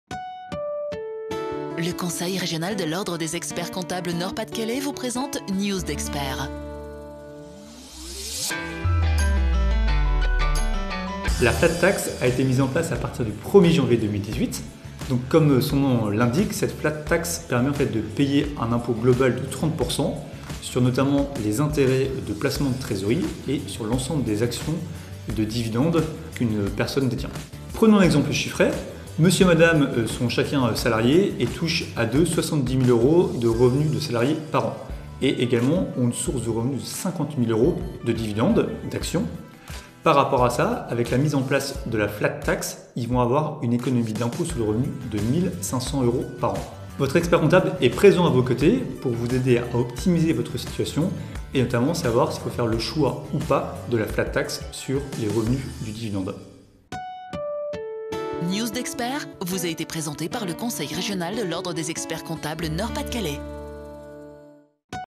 News d'experts: la flat tax
Focus sur la Flat Tax dans ce nouveau numéro de "News d'experts" sur Grand Littoral TV.